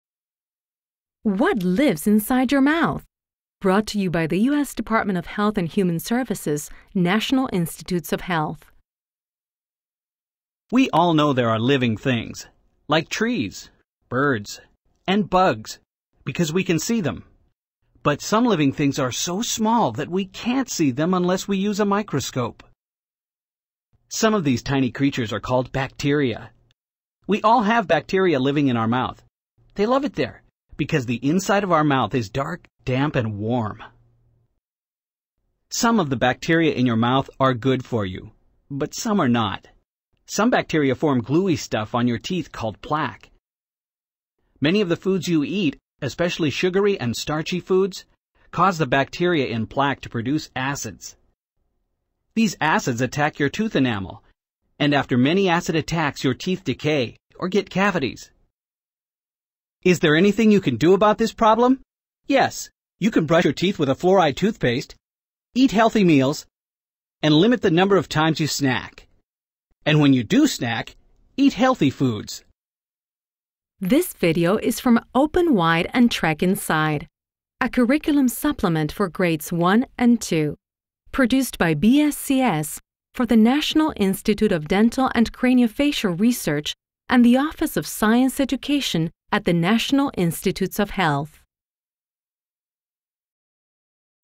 NIDCR: What Lives Inside Your Mouth?
For children in grades 1 and 2, this video explains how bacteria in our mouths use sugars in food to produce acids that attack tooth enamel and cause tooth decay.

For more information about the National Institute of Dental and Craniofacial Research,